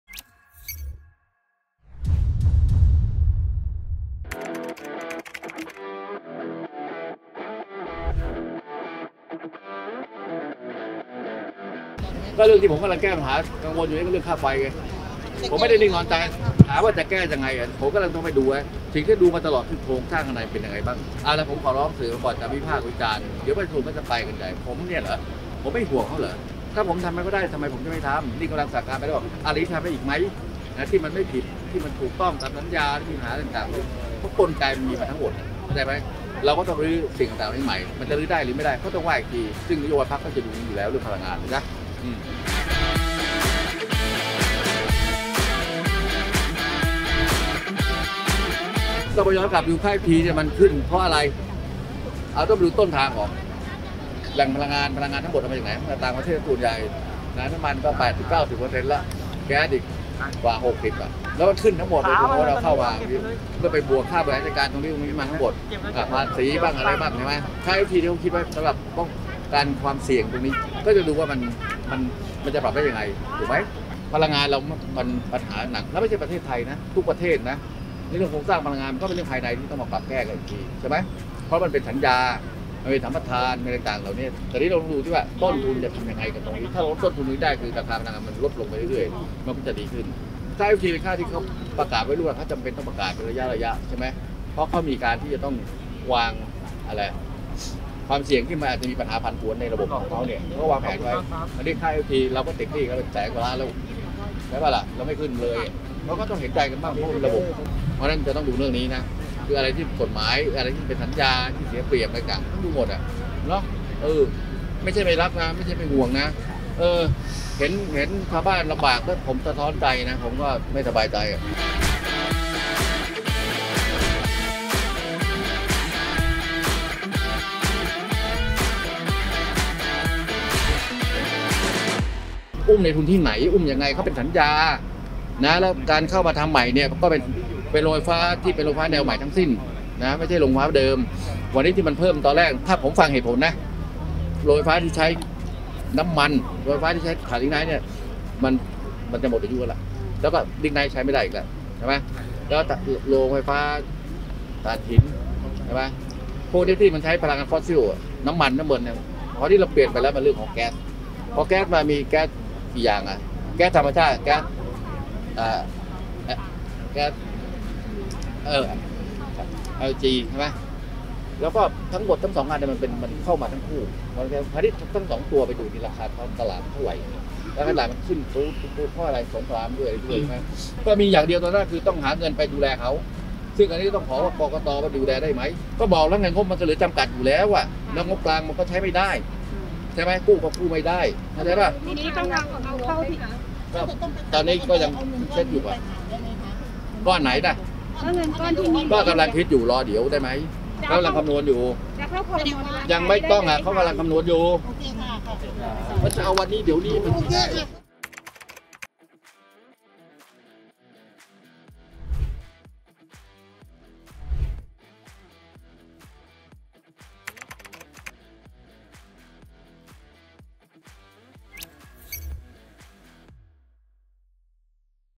รัฐบาล ‘พล.อ.ประยุทธ์’ ตัวการค่าไฟแพง กับวิธีแก้ปัญหาแบบสุดโต่ง | THANDIGITAL
รัฐบาล ‘พล.อ.ประยุทธ์’ อยู่มา 8 ปี จัดการอย่างไรกับค่าไฟที่แพงขึ้น?

ระหว่างลงพื้นที่หาเสียงที่สวนลุมพินี
เผยว่า กังวลเรื่องค่าไฟ และไม่ได้นิ่งนอนใจ
กำลังหาทางช่วยเหลือ
ผุดไอเดียให้ประชาชนใช้ไฟน้อยลง ค่าไฟจะถูกลง
ยึดหลัก “ปิด-ปรับ-ปลด-เปลี่ยน”